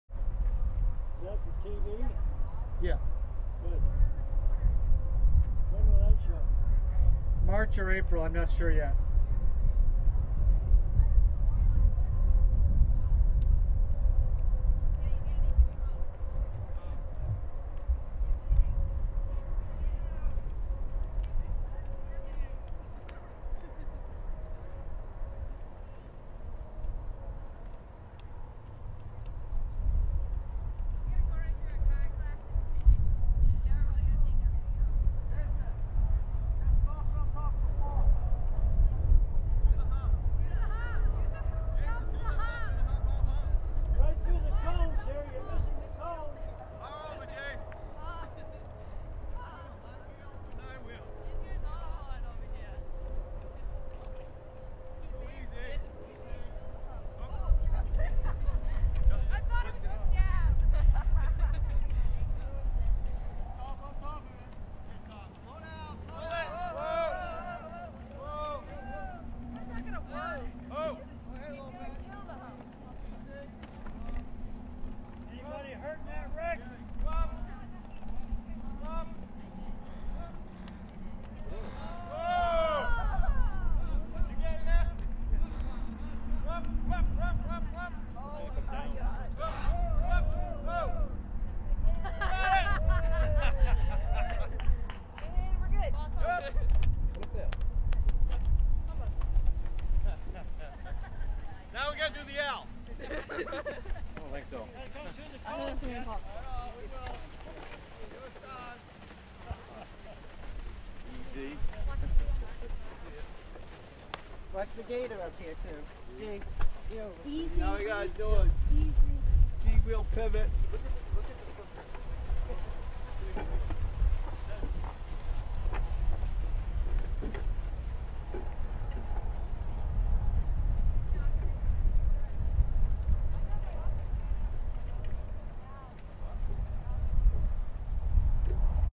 Maine Oxen at Kentucky Horse Park
Maine Oxen Teamsters showcase their cattle at the Kentucky Horse Park in October 2011. Here they put eight teams to a "big wheel" logging arch. Within a couple of days they were driving this hitch with one teamster.